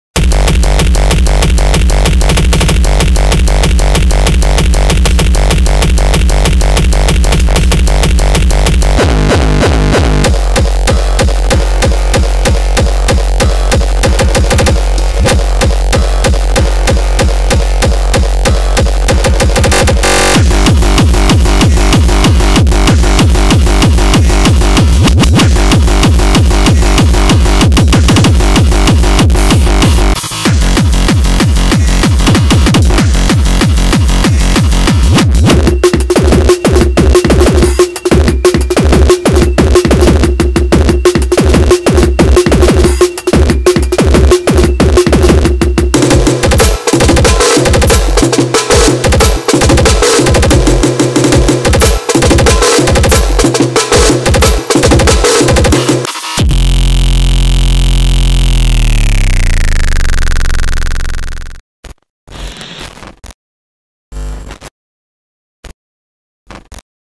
Frenchcore Samples Pack 02 By Dam V2
CONTENT :
5 Drums loops
10 Hit Hat loops
32 Kicks one shot
10 Noise
10 snares one shot